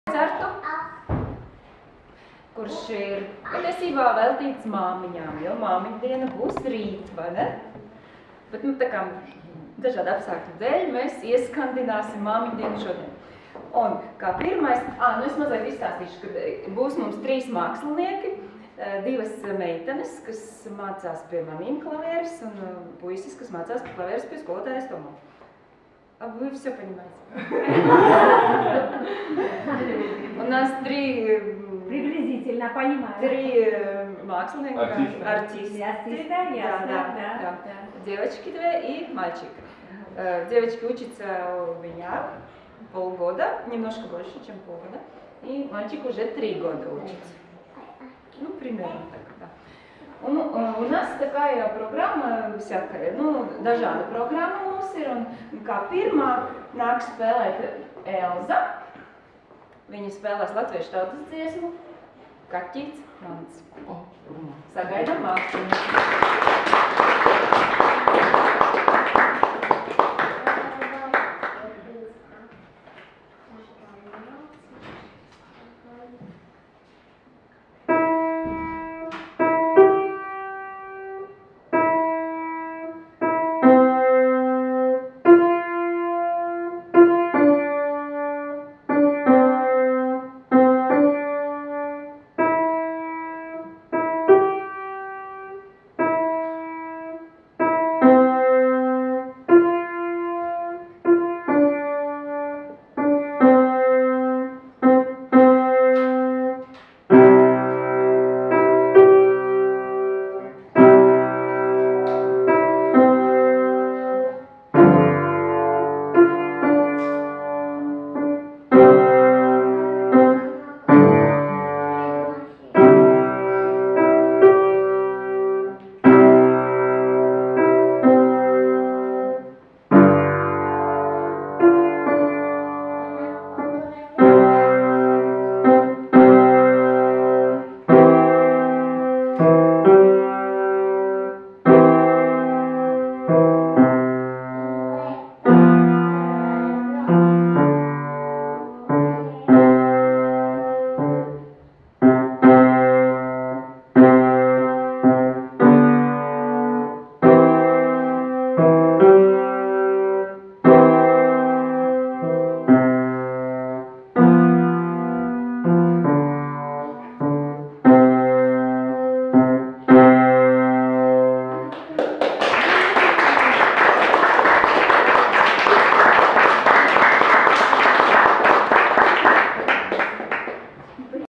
Elza Dreimane
bērnu klavierkoncerts māmiņu dienā, Tbilisi 2015